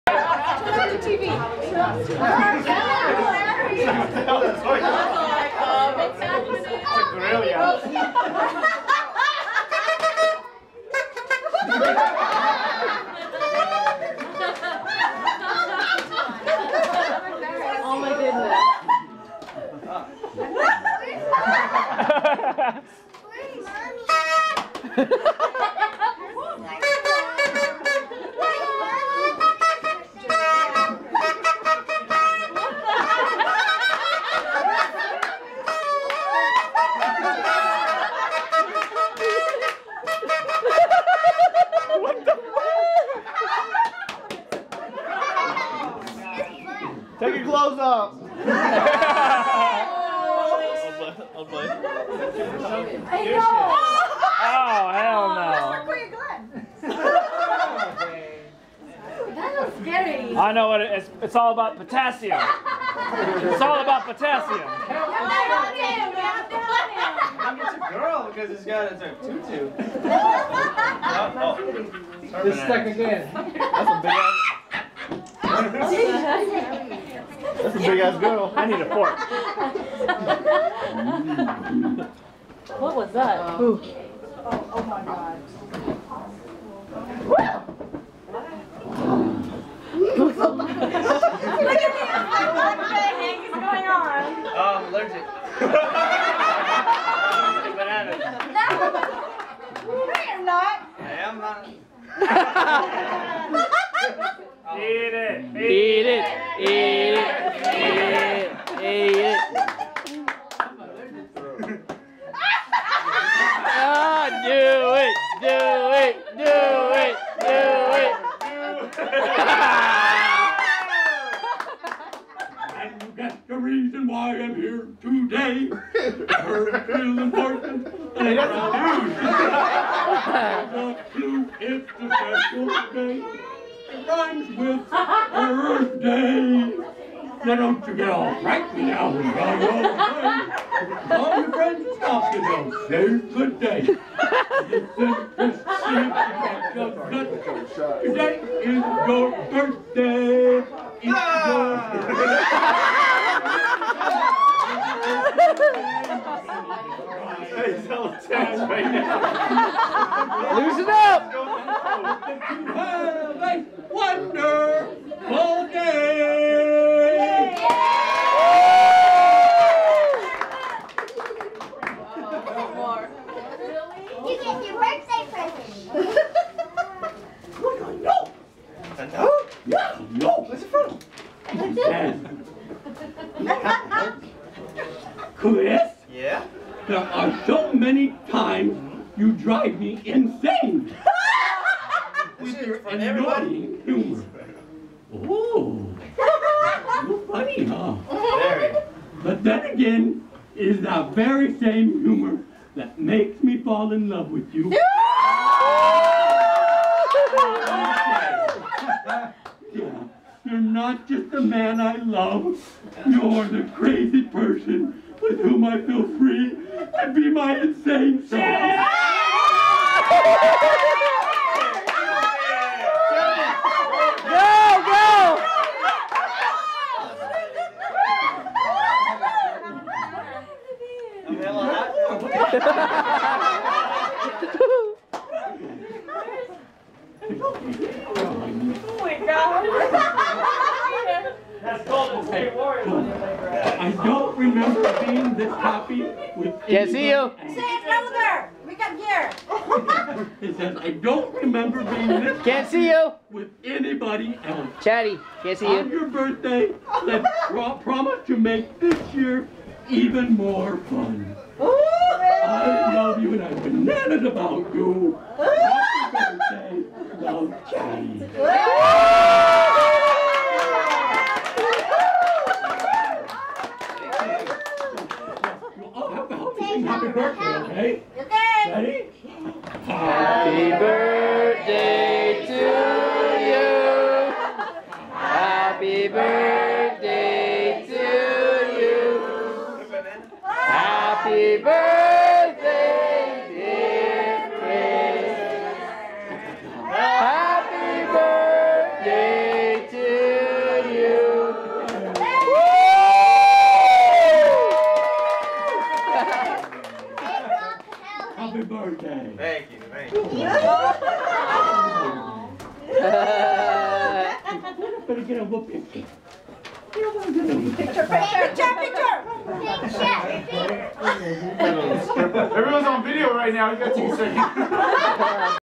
Singing Gorilla for my Birthday
Chatty surprises me with this crazy ass gorilla on my 30th birthday.  He reads me a note that she wrote for me professing her love to me in front of all of my friends and family.